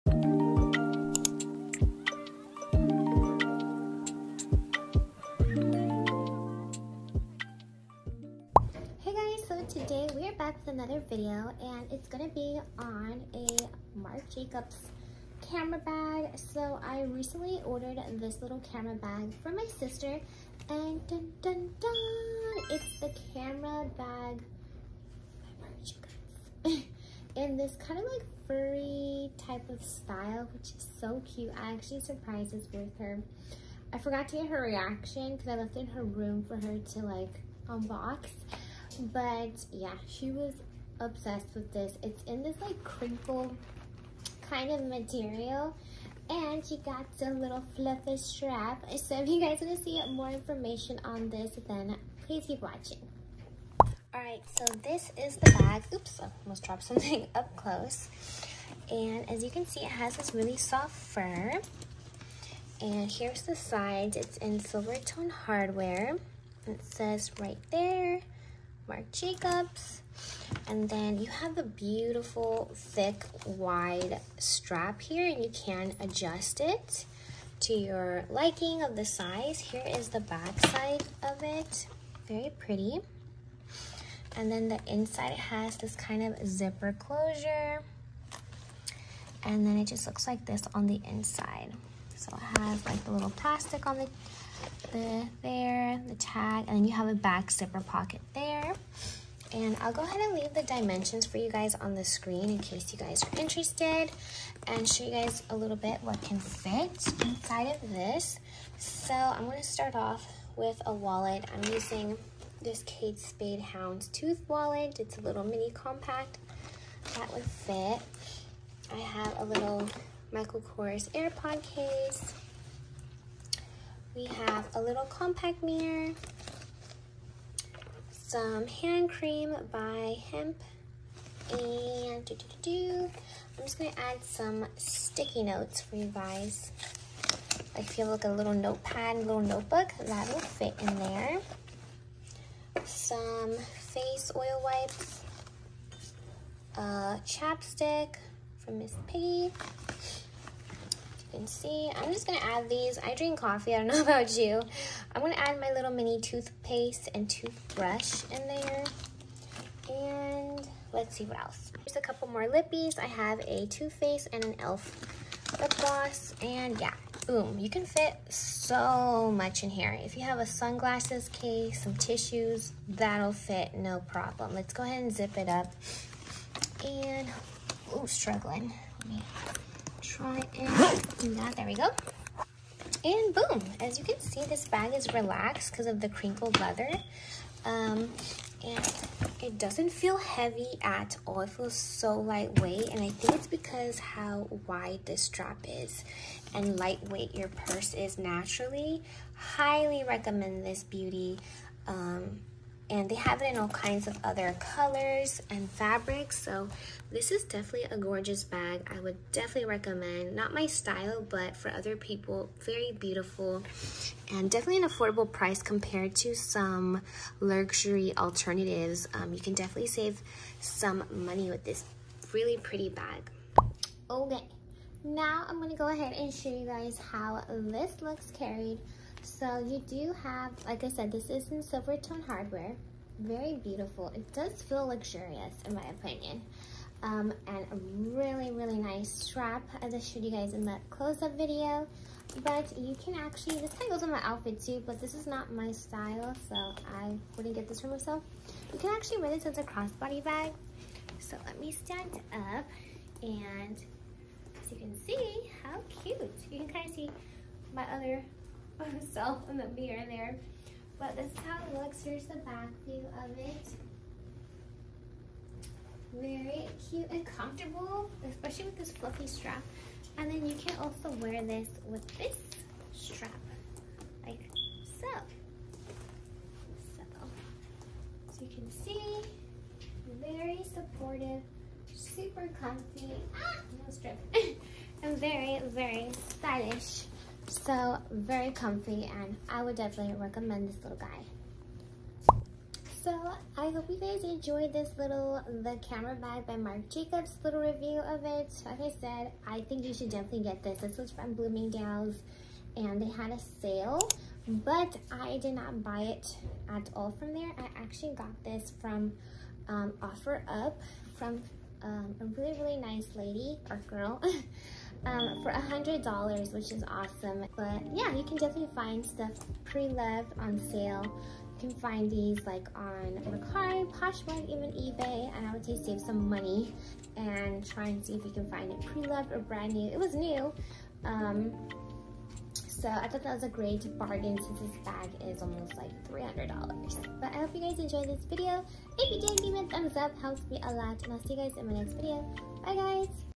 MARC JACOBS THE CRINKLE SHEARLING CAMERA BAG REVIEW | MOD SHOTS | WHAT FITS?
Hello 🌹
Today is a mini review of the Marc Jacobs Crinkle Shesrling Camera Bag. This is a beauty! May not be everyone’s style but definitely an eye catching piece to any outfit.